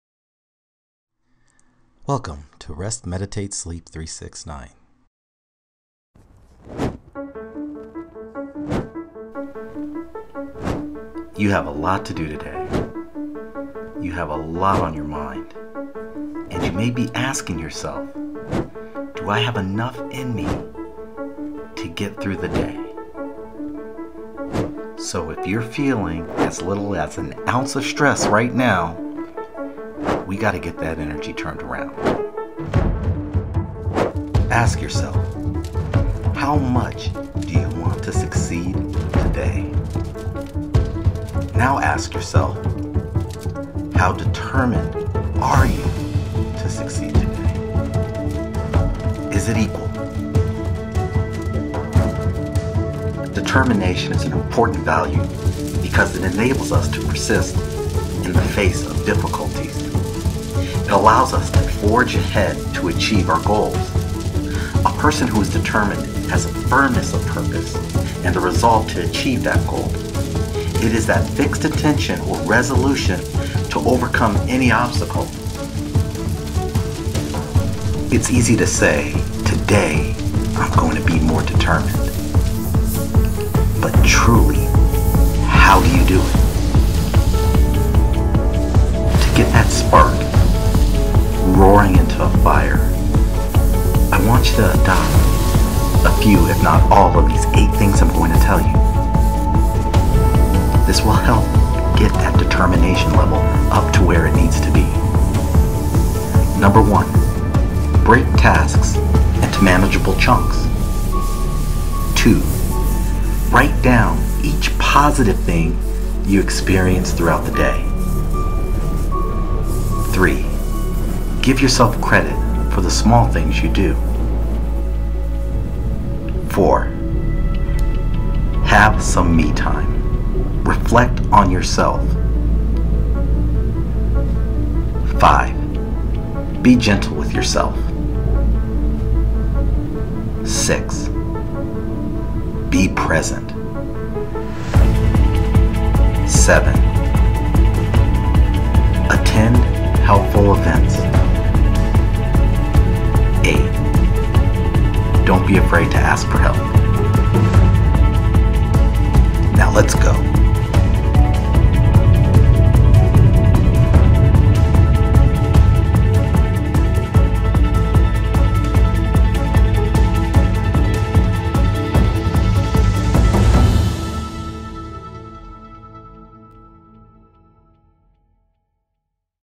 Determination: How to get daily tasks done to get a good night's sleep
In this video, I'm sharing tips on how to get daily tasks done to get a good night's sleep. This will help you to be more determined, achieve your goals and sleep soundly!

As a busy person, stress is not something you can avoid. Understanding that tasks that are left undone weigh on your mind and that weight prevents you from slowing your brain down at night. You can make sure that you get enough sleep each night by being productive each day. I hope these simple tips, spark a fire of determination.
Let's Go